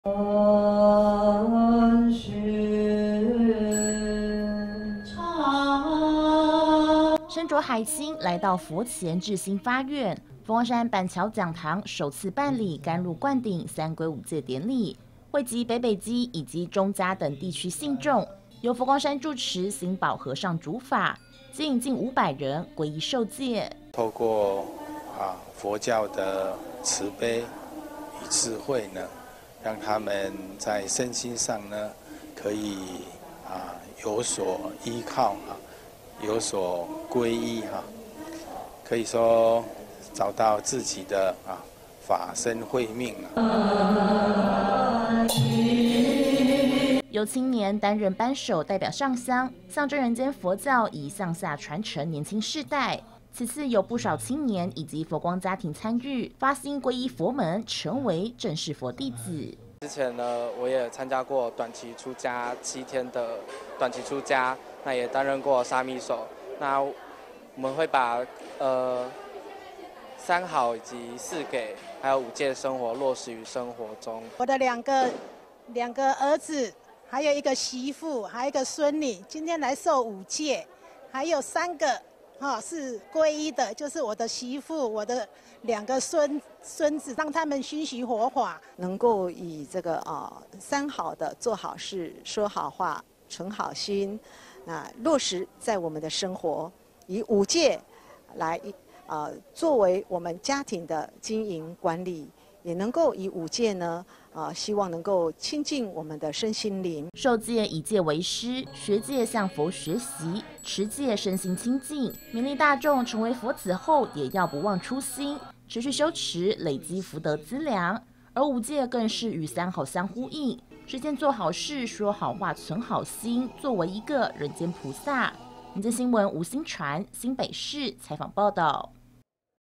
20240718板橋講堂首辦三皈五戒 心保和尚主法證盟皈依｜@FGSBLTV
佛光山板橋講堂，日前首度舉辦「甘露灌頂三皈五戒典禮」，近500名信眾前來受戒，由佛光山住持心保和尚主法證盟皈依，而此次班首均由青年擔任，象徵人間佛教信仰傳承。